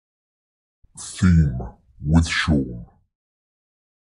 THEME with Shawn, opening titles in 1080p
From July 2023, music playlists with a theme will use this title. The Weekend Starts Here is no more. Long live THEME.